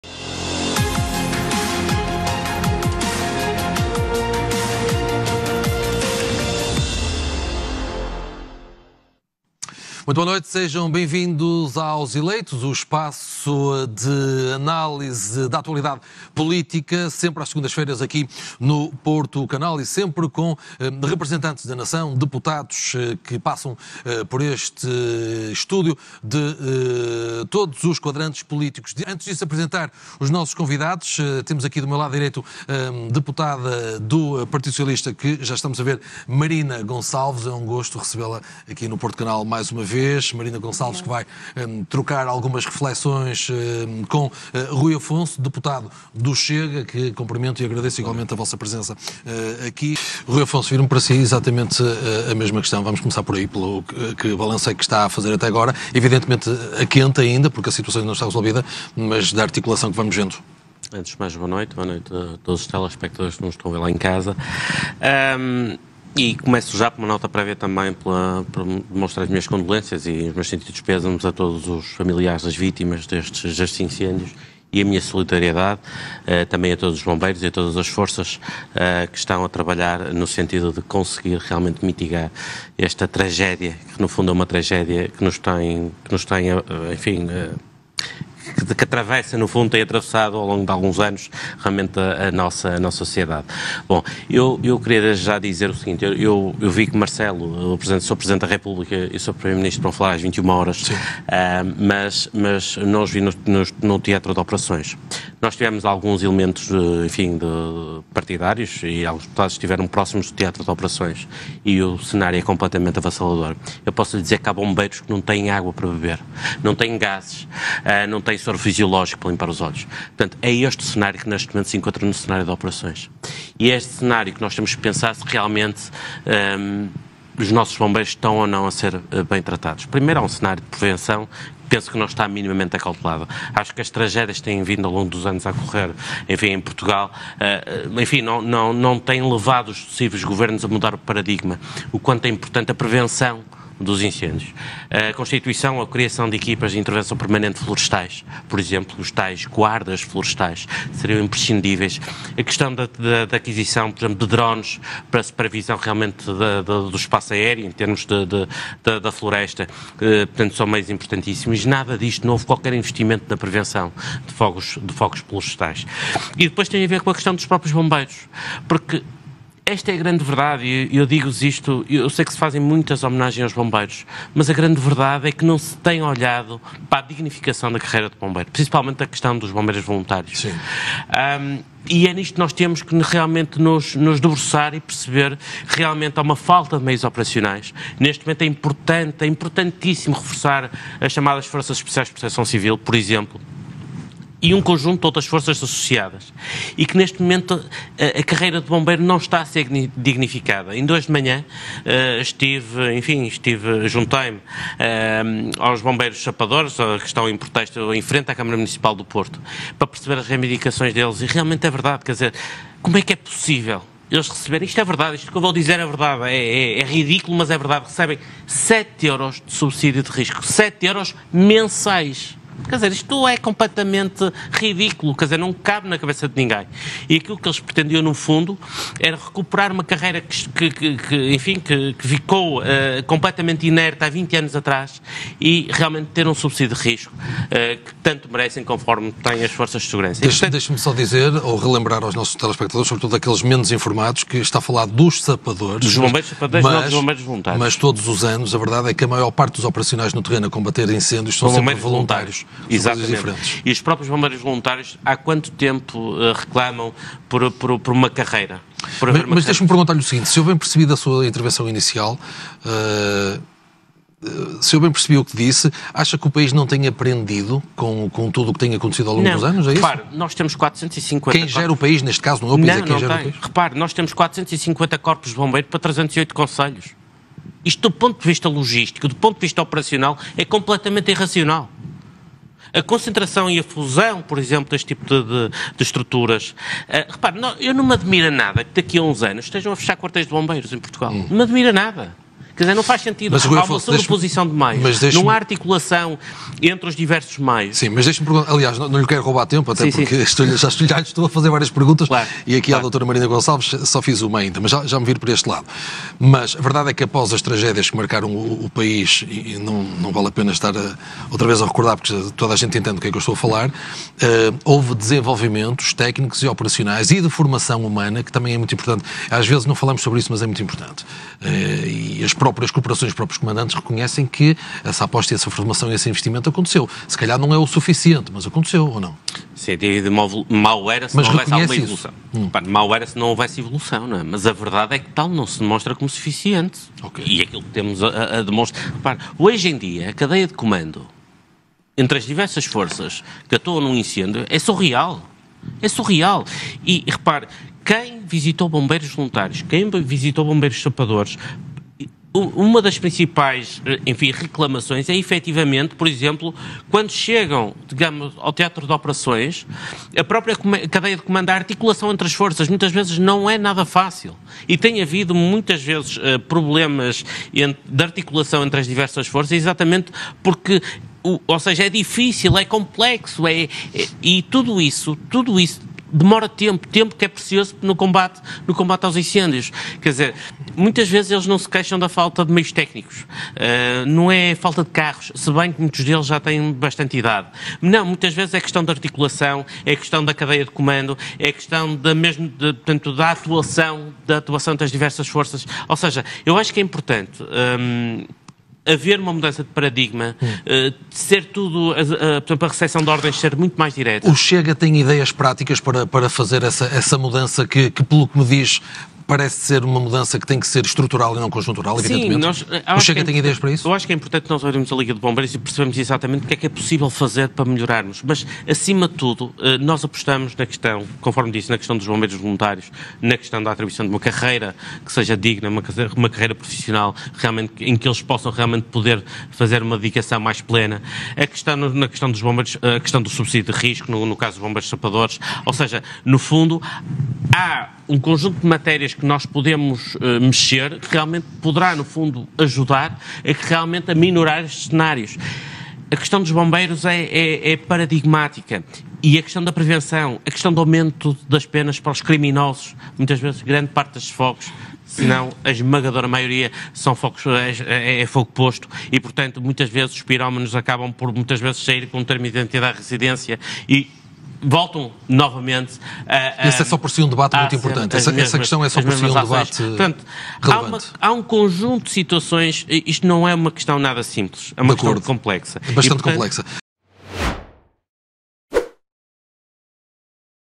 "Os políticos só se lembram dos bombeiros quando há fogos"
A intervenção do deputado Rui Afonso em debate no Porto Canal.